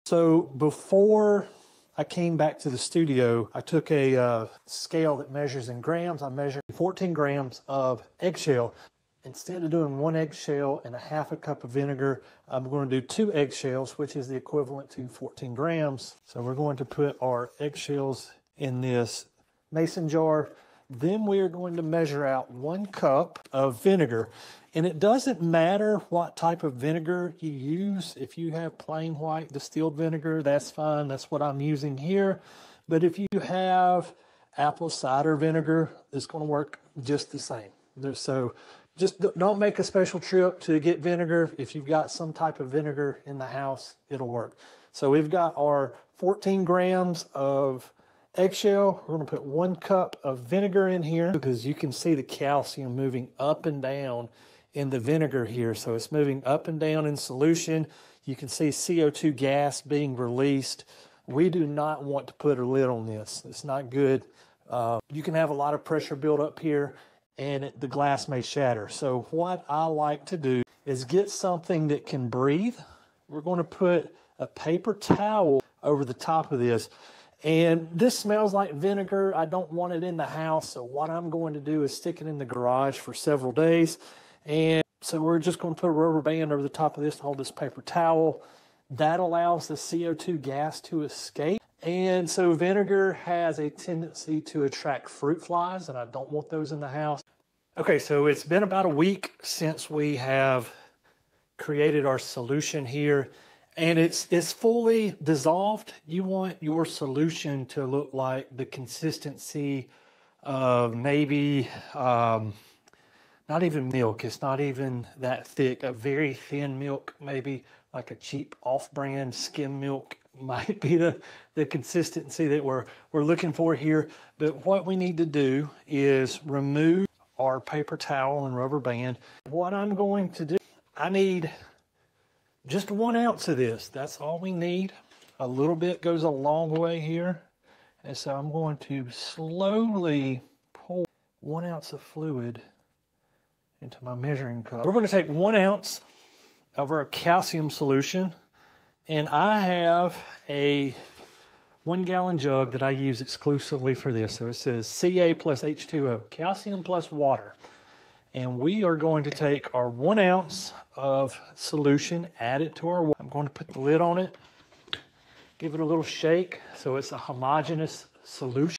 Transforming Eggshells into Fertilizer
Don't throw away your eggshells! Turn them into FREE, organic fertilizer for your plants. This video shows you a simple, step-by-step method to transform eggshells into a calcium-rich powder that your plants will love. Improve your garden's health naturally and reduce waste